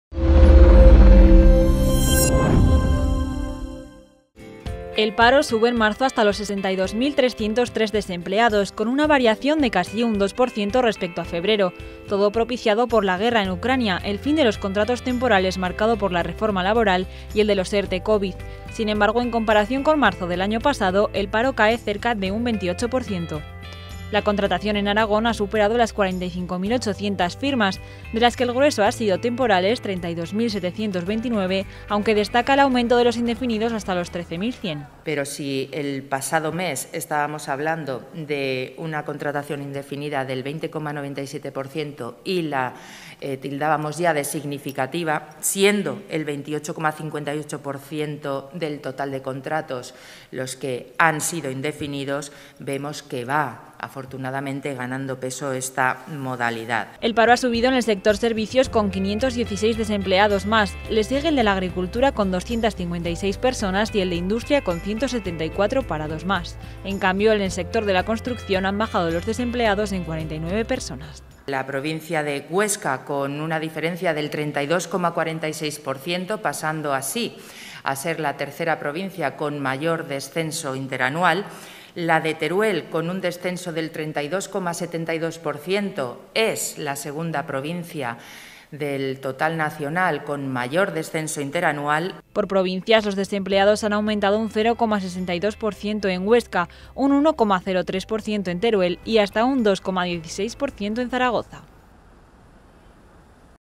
La guerra en Ucrania, el fin de los ERTE Covid y de contratos temporales engrosan el paro aragonés
El paro en Aragón ha subido en 1.140 personas en marzo, pero ¿de dónde provienen estos parados? Según la consejera de Economía, Marta Gastón, se han dado varias circunstancias este mes que han provocado que haya aumentado el desempleo de la Comunidad. Estas son la incertidumbre en el mercado causada por la guerra en Ucrania, la fecha del día 31 de marzo como fin de los contratos temporales según marca la reforma laboral y también ese mismo día como finalización de las prórrogas de los ERTE Covid. Todo ello ha contribuido a que las cifras del paro aragonés hayan ascendido un 1,86% en comparación con el mes de febrero.